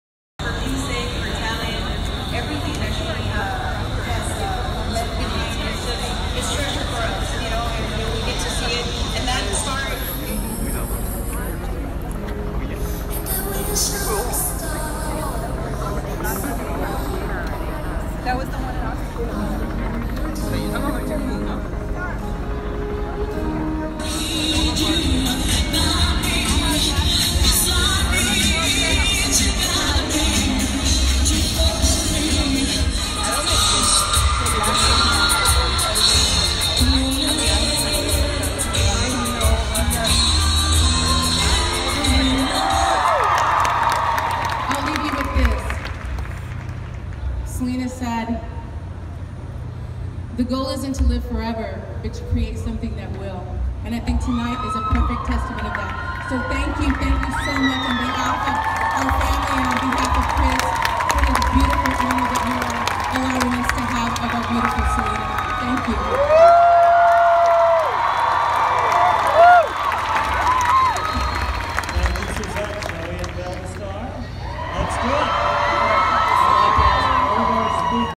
Selena Quintanilla Hollywood Walk of Fame star ceremony. November 3, 2017
The late Selena Quintanilla Hollywood Walk of Fame star last night in front of the Capital Records building on Vine Street. The sister of Selena Suzette Quintanilla was a special guest speaker. Filmed by AlexC43. Los Angeles, CA. November 3, 2017